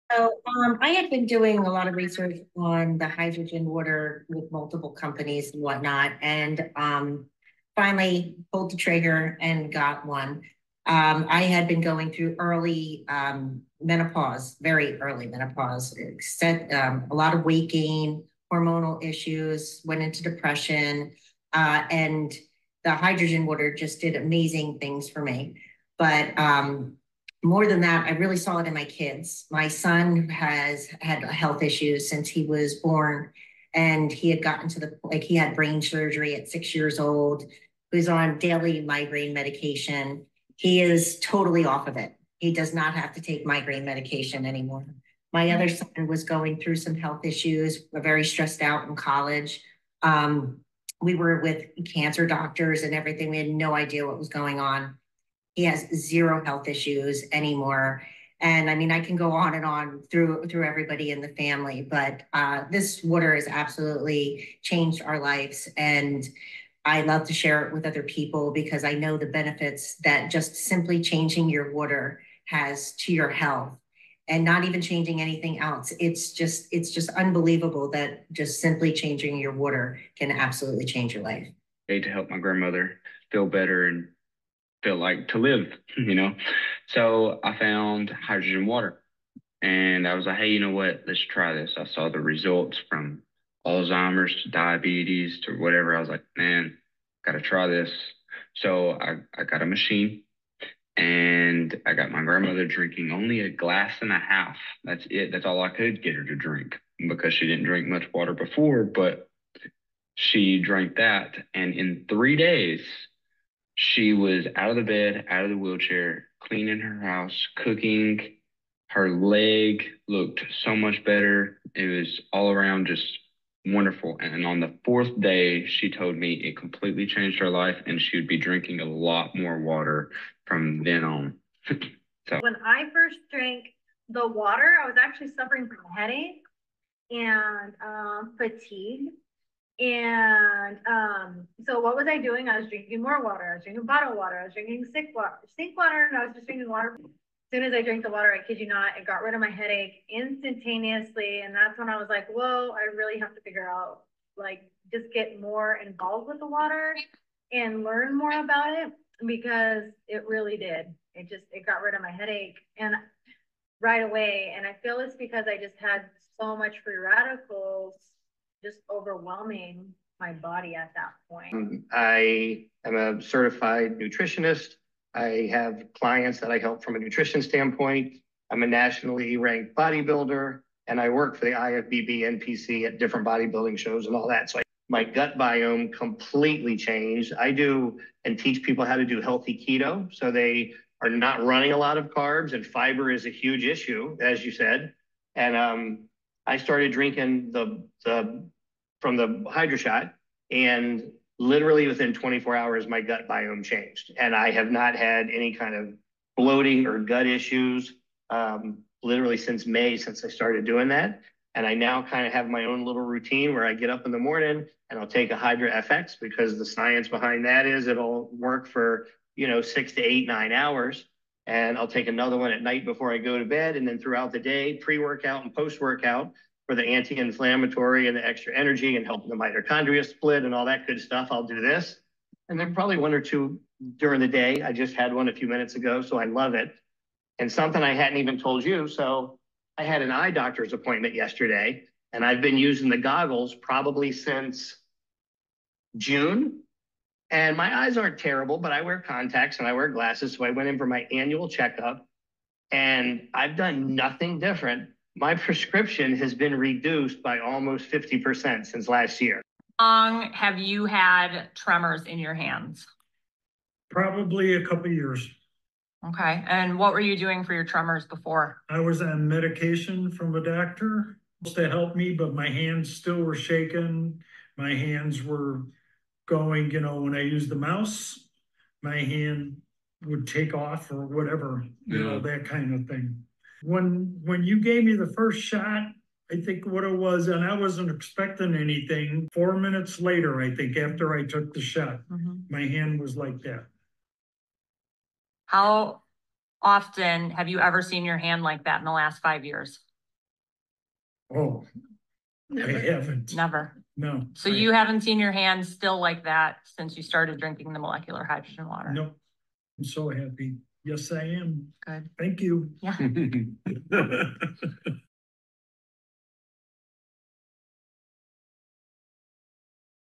Hydrogen Water Changed our Lives | Healthy Hydration
In this video, I share my incredible journey with hydrogen water and how it has transformed my life and the lives of my loved ones. From overcoming early menopause symptoms to helping my son recover from brain surgery and supporting my grandmother's health, hydrogen water has been a true miracle.

I'll dive into the science behind hydrogen water, explaining how it works to reduce oxidative stress and promote cellular health. You'll hear firsthand accounts of how hydrogen water has helped with various health issues, including:

Chronic pain
Digestive problems
Skin conditions
Mental clarity
Energy levels
I'll also share my personal tips and routines for incorporating hydrogen water into your daily life. Whether you're a health enthusiast or simply looking to improve your well-being, this video will inspire you to explore the amazing benefits of hydrogen water.

Key takeaways:

Hydrogen water is a powerful antioxidant
It can improve overall health and well-being
It's easy to incorporate into your daily routine
Don't miss this opportunity to learn more about this incredible natural remedy!